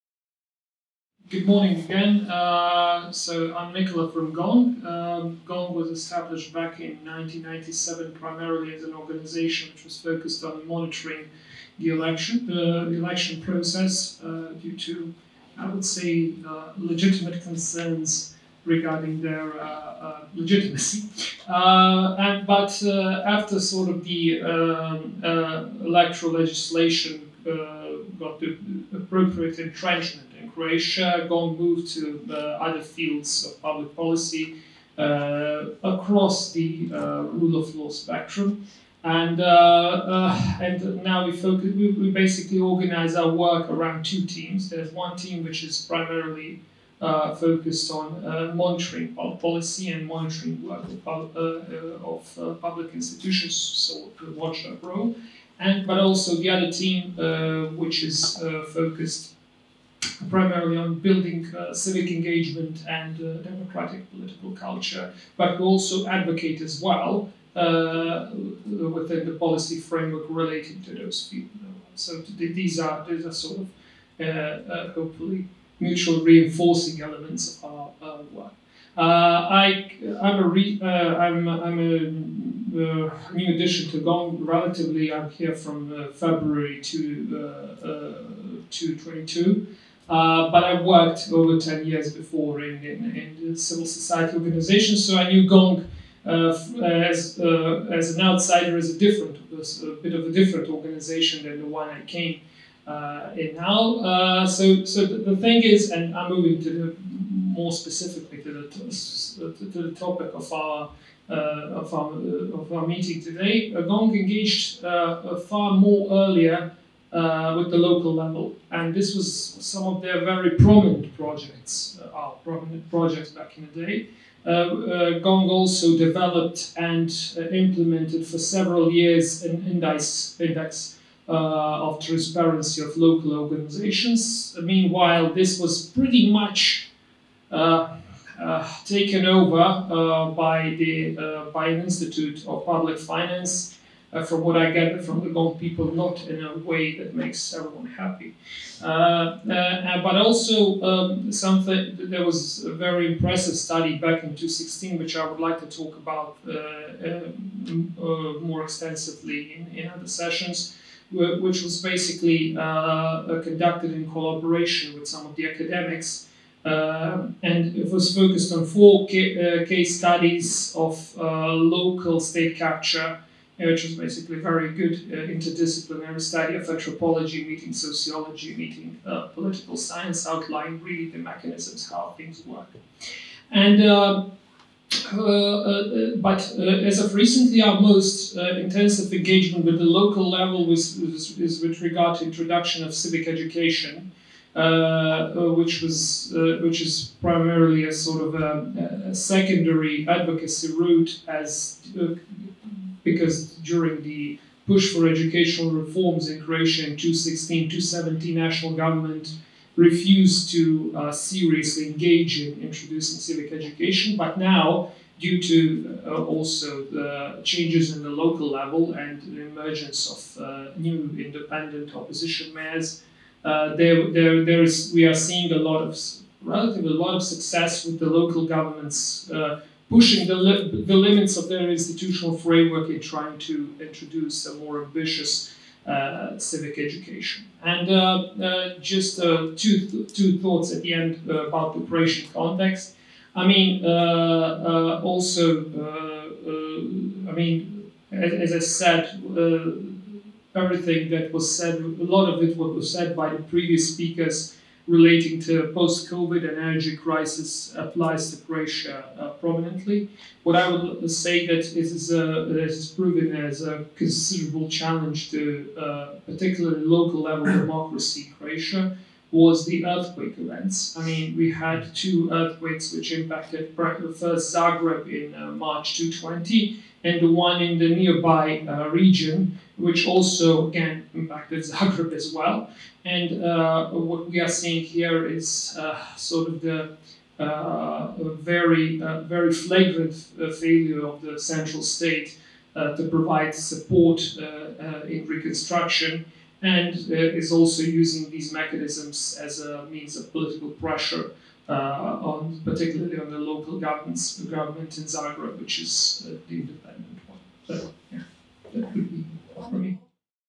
Nikola Buković - Gong | Conspiracy for Democracy 2022
Conspiracy for DEMOCRACY! 👁 🤫✨
10 ország aktivistái gyűltek össze Budapesten azért, hogy civil szervezetek, kezdeményezések és önkormányzatok hatékony együttműködésének lehetőségeiről gondolkodjanak közösen. Közös célunk, hogy a COVID-19 járvány nehézségei és Oroszország ukrajnai inváziójának következményei közepette egy nyitottabb és demokratikusabb helyi közélet kialakítása.

Az eseményt a K-Monitor szervezte.